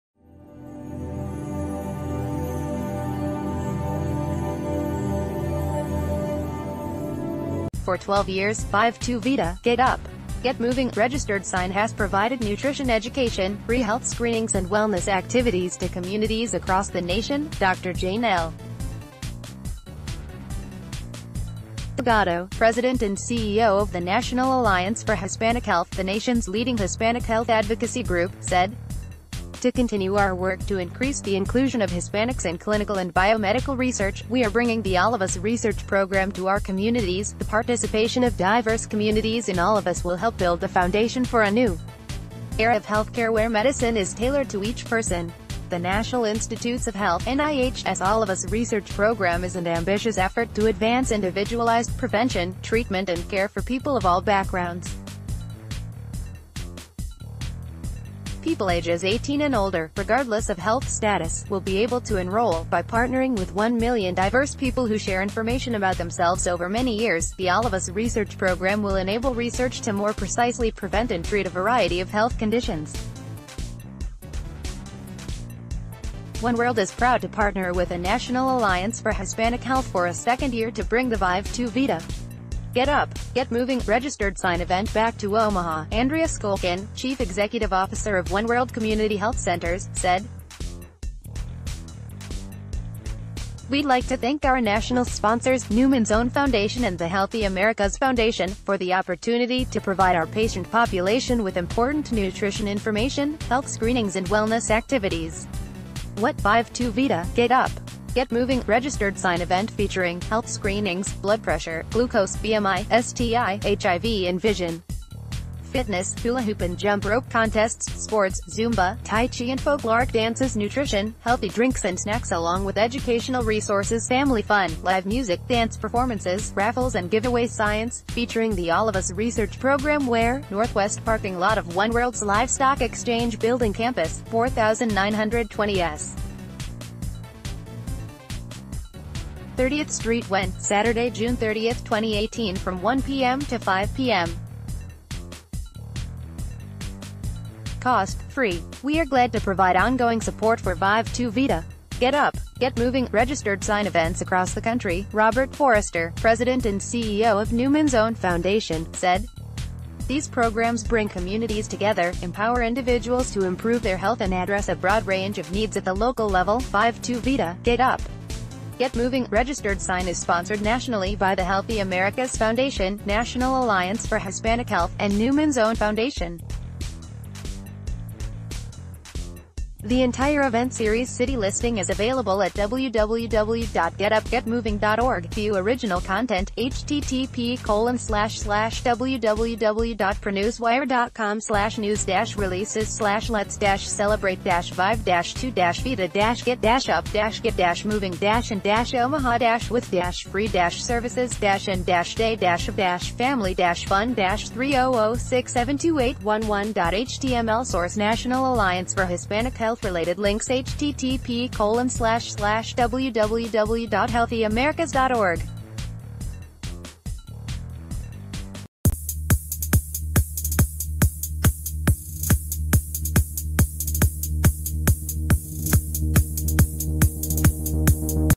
Breaking News | Let's Celebrate ¡Vive Tu Vida! Get Up! Get Moving!® in Omaha with Free Services and
Breaking News | Let's Celebrate ¡Vive Tu Vida! Get Up! Get Moving!® in Omaha with Free Services and Day of Family Fun

"For twelve years, ¡Vive tu vida! Get Up! Get Moving! ® has provided nutrition education, free health screenings and wellness activities to communities across the nation," Dr. Jane L. Delgado, President and CEO of the National Alliance for Hispanic Health, the nation's leading Hispanic health advocacy group, said. "To continue our work to increase the inclusion of Hispanics in clinical and biomedical research, we are bringing the All of Us Research Program to our communities. The participation o...